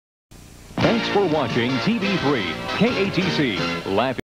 KATC Station ID 1988 #3
This is KATC's third station ID from 1988
KATC is located in Lafayette, LA